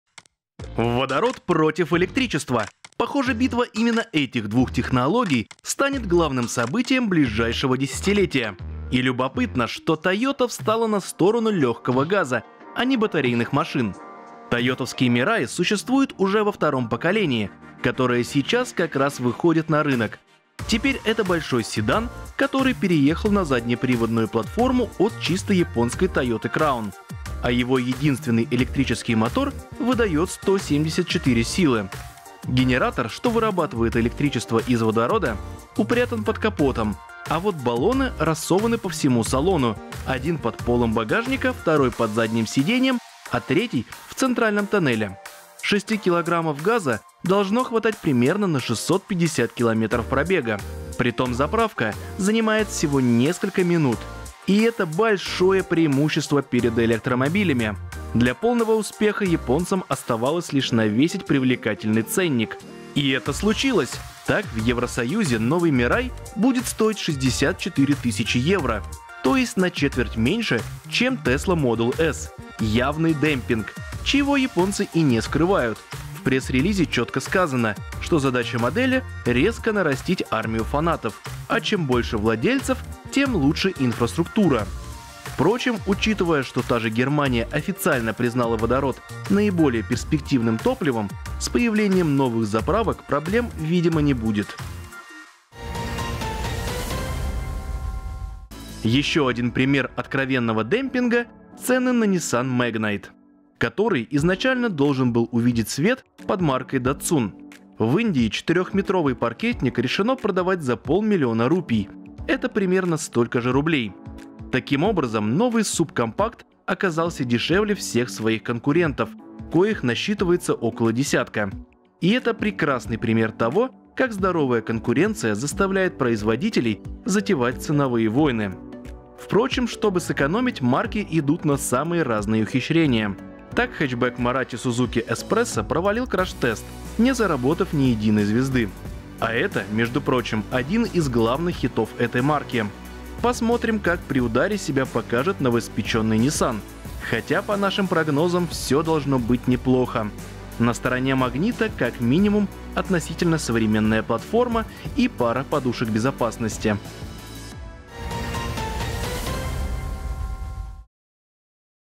Toyota Mirai не побоялась демпинговать. Nissan Magnite за полмиллиона рублей | Новости с колёс №1269
Водород против электричества — похоже, битва именно этих двух технологий станет главным событием ближайшего десятилетия. И любопытно, что Тойота встала на сторону лёгкого газа, а не батарейных машин. Тойотовский Мираи существует уже во втором поколении, которое сейчас как раз выходит на рынок. Теперь это большой седан, который переехал на заднеприводную платформу от чисто японской Тойоты Crown, а его единственный электрический мотор выдаёт 174 силы.

Ещё один пример откровенного демпинга — цены на Nissan Magnite, который изначально должен был увидеть свет под маркой Datsun. В Индии четырёхметровый паркетник решено продавать за полмиллиона рупий — это примерно столько же рублей. Таким образом, новый субкомпакт оказался дешевле всех своих конкурентов, коих насчитывается около десятка.